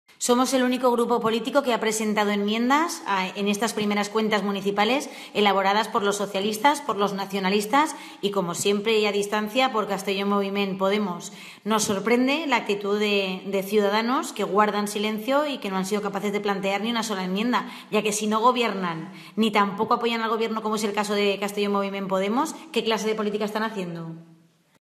Ciudadanos no presentó ninguna enmienda al presupuesto municipal de Castellón
El Grupo Municipal Popular ha manifestado hoy que el proceso de elaboración y debate de los Presupuestos municipales para 2016 está poniendo de manifiesto que “el PP es la única oposición constructiva y responsable”.